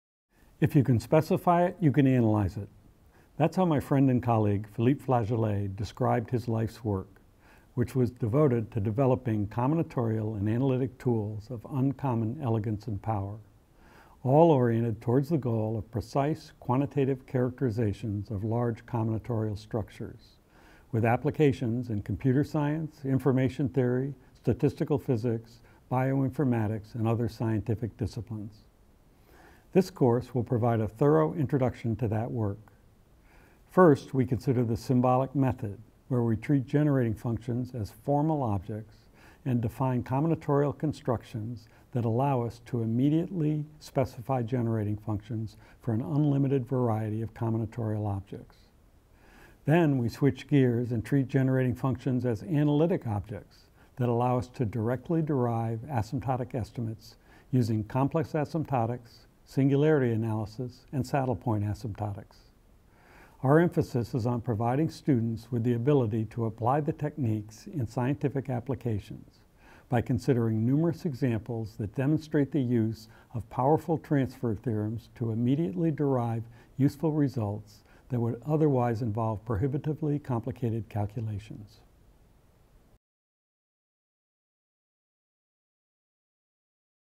Analytic Combinatorics
Analytic Combinatorics teaches a calculus that enables precise quantitative predictions of large combinatorial structures. This course introduces the symbolic method to derive functional relations among ordinary, exponential, and multivariate generating functions, and methods in complex analysis for deriving accurate asymptotics from the GF equations.